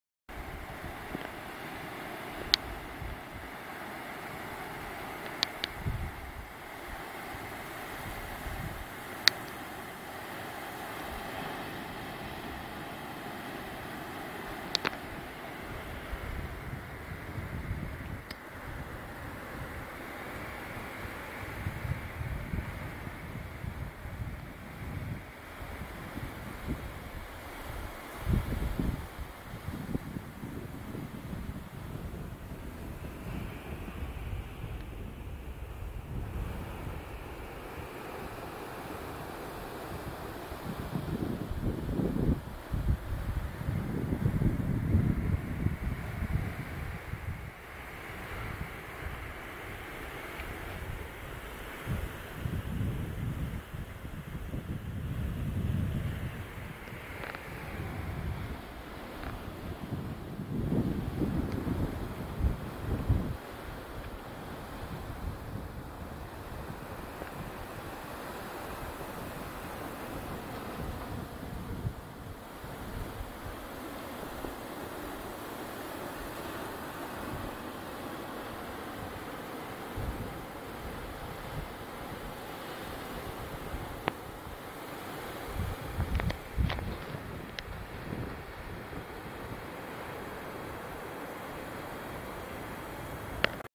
Another Beach in New Tolsta, Island of Lewis, Scotland.
Breathtaking drop at another beach in New Tolsta. Taken on the Island of Lewis in Scotland early 2009. Western Isles.